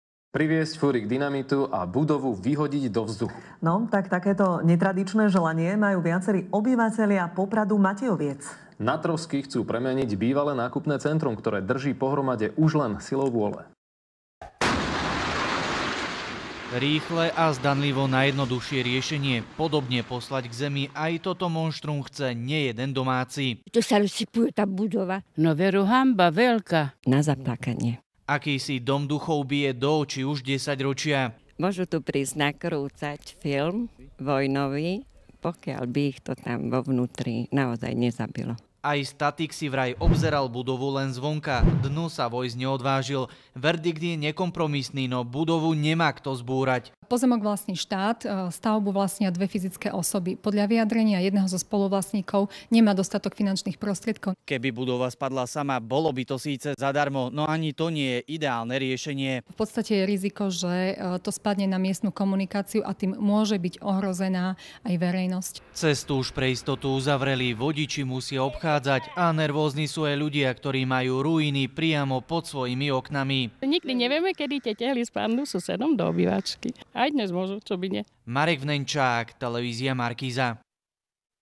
MATEJOVCE: STRAŠIAKA NEMÁ KTO ZBÚRAŤ
Bývalé nákupné centrum drží pohromade už len silou vôle